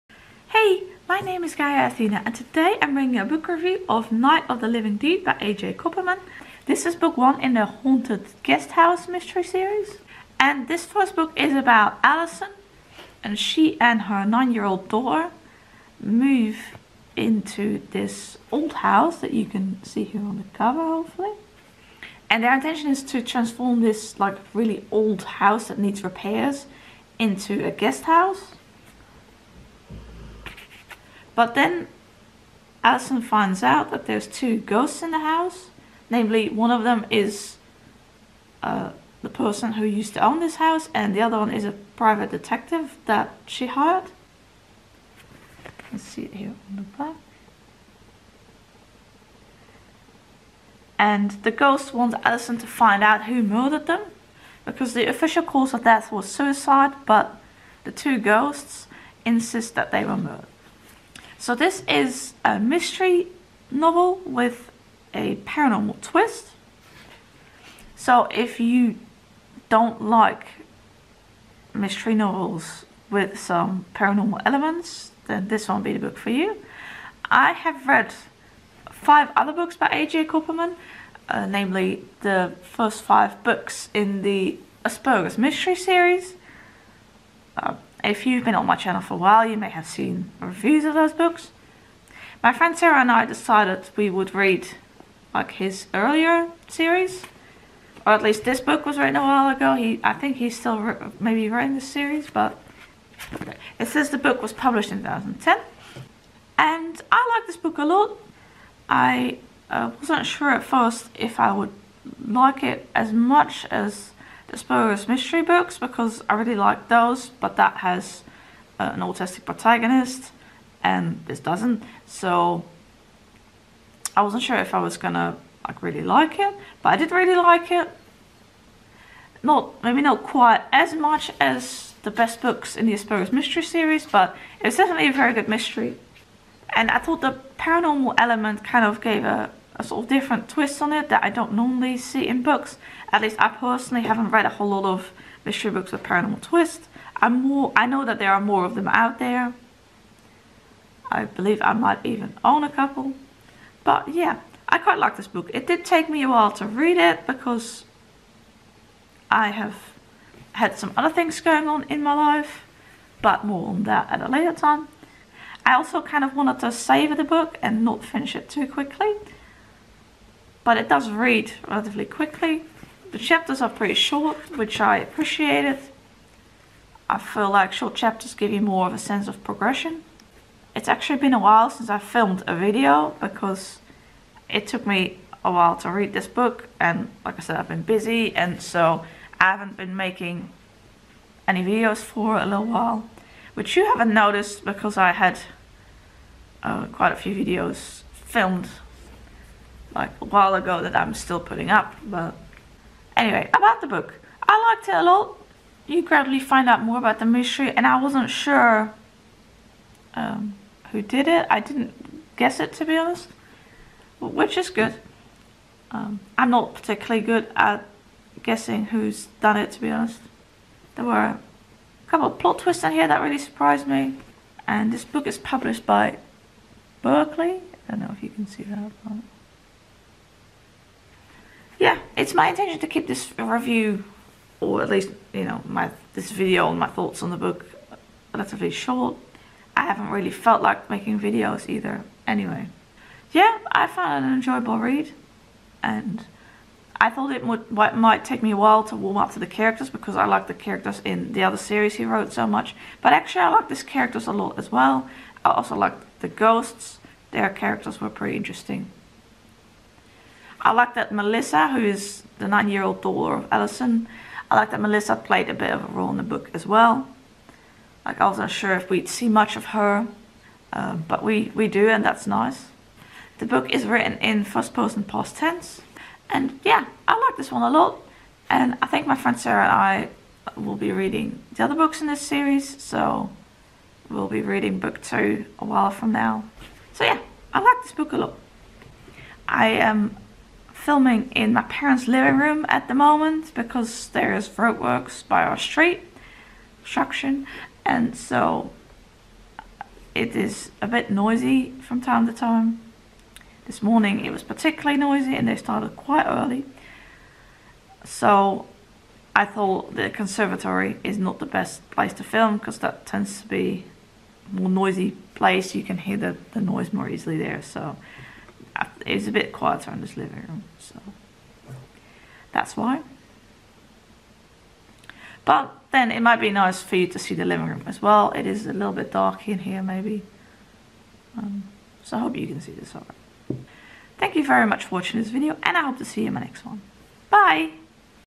Book Review: Night of the Living Deed by E. J. Copperman
In this video I share some bookish thoughts on Night of the Living Deed (Haunted Guesthouse Mystery #1) by E. J. Copperman.